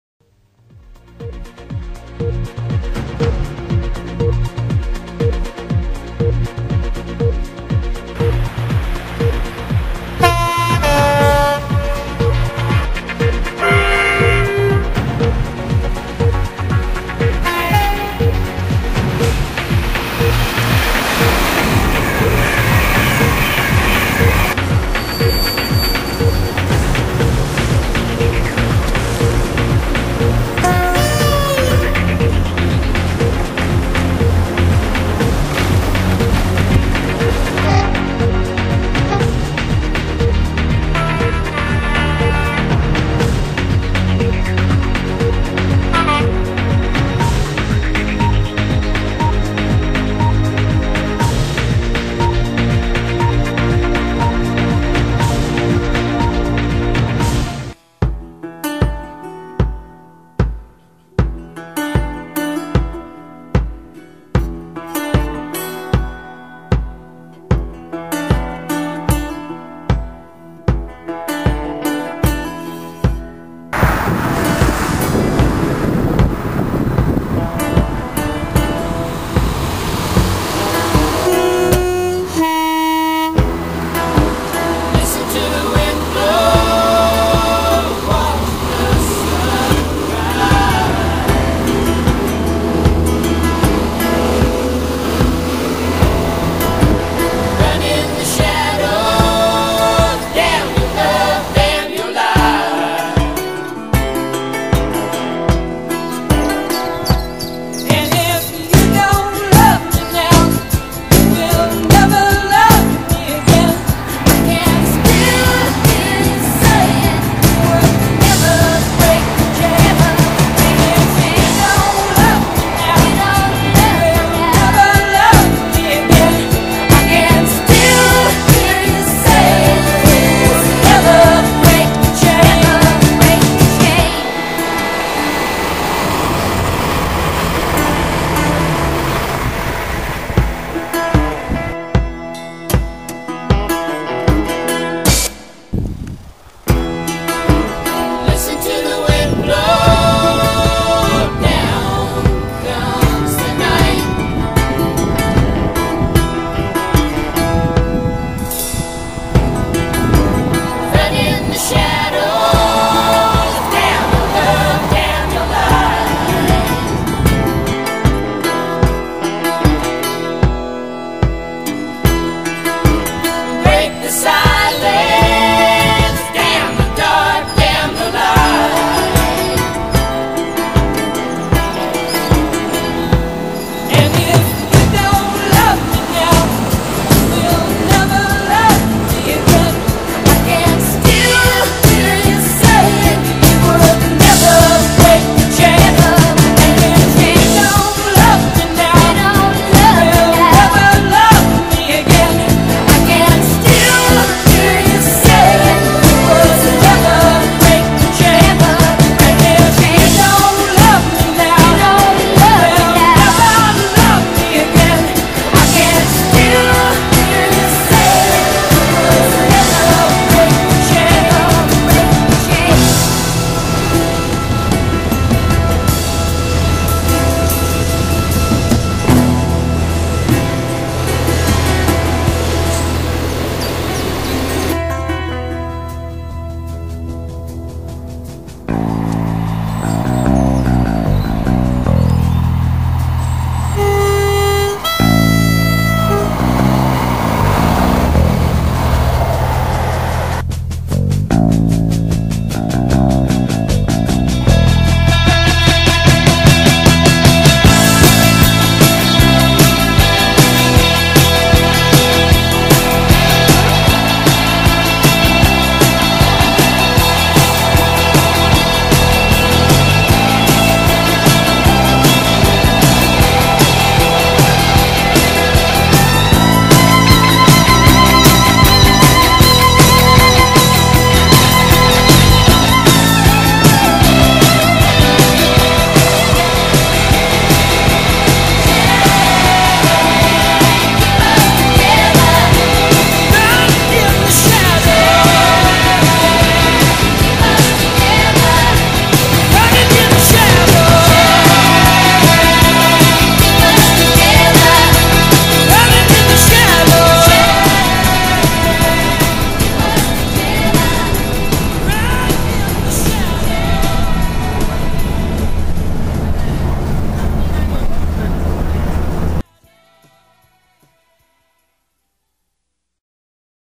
Ram Hill Bridge - The Movie
A selection of videos froim a recent trip to Ram Hill footbridge